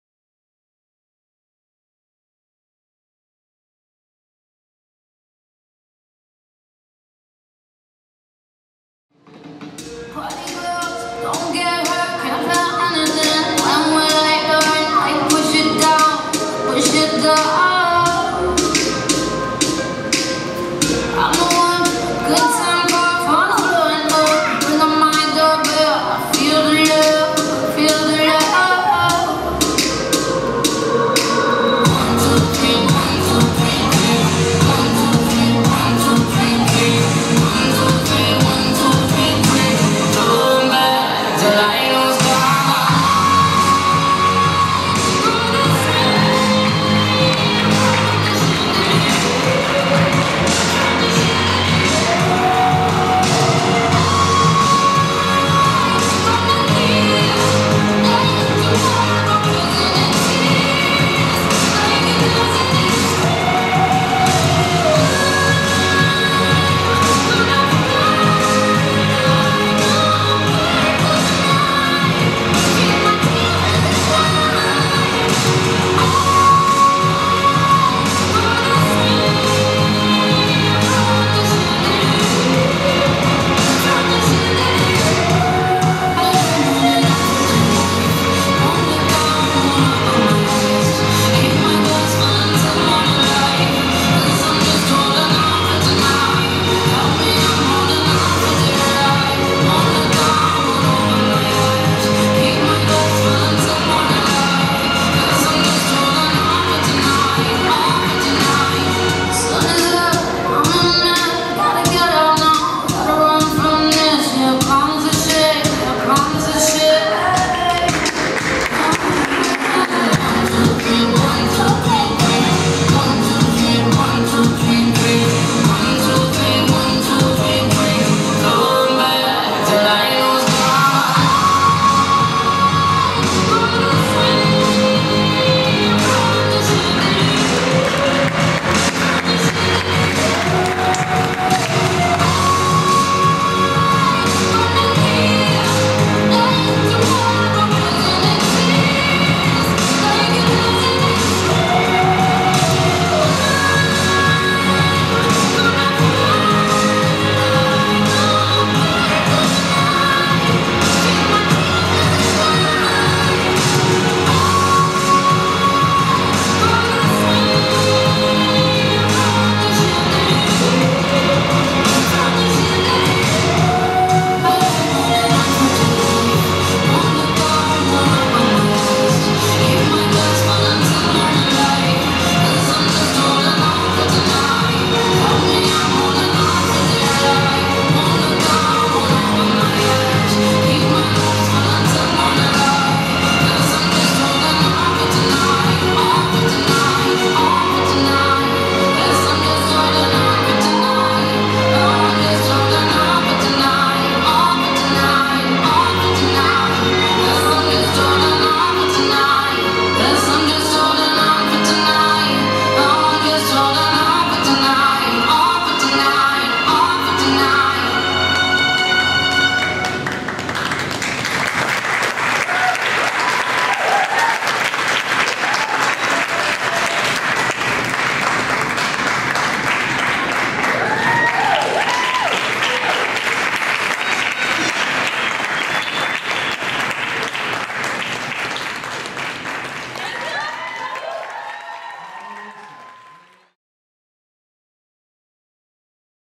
Lisa Sangiorgio and Mary Brumbaugh - 6-27-2014
Lisa Sangiorgio and Mary Brumbaugh perform on the silks at Cirque Indy's Student Showcase